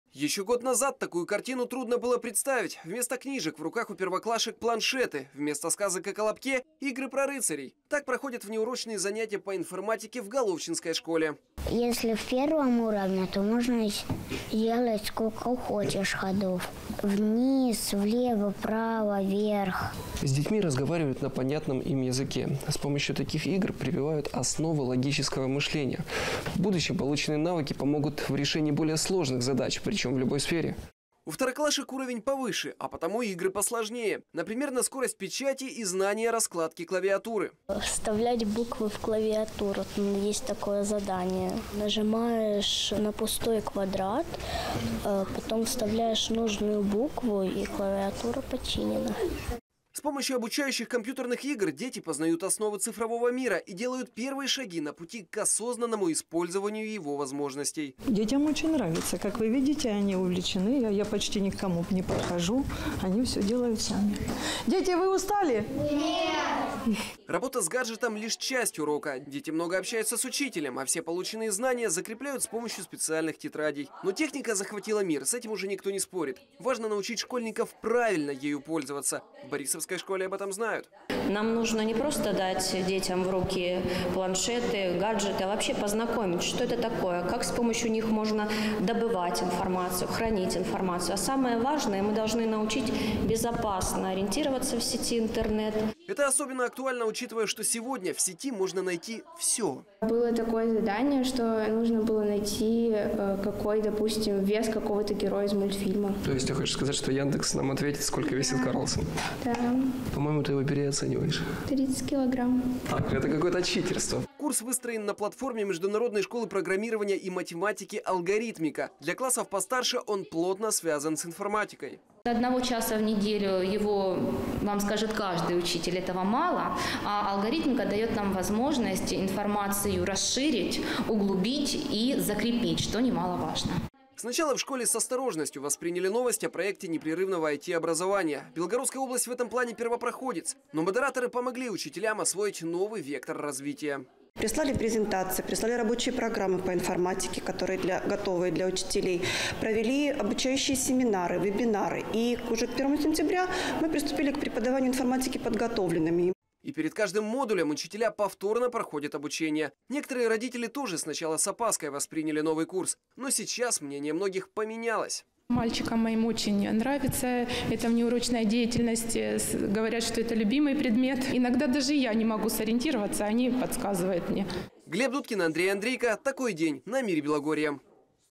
Как проходят IT-уроки в белгородских школах
С этого учебного года в Белгородской области стартовал проект непрерывного IT-образования. Школьники с первого класса постигают цифровой мир. В этом новом формате учебные учреждения работают уже месяц.

Наши журналисты побывали на нескольких уроках, чтобы увидеть, как всё выглядит на практике. Глеб Дудкин расскажет подробности.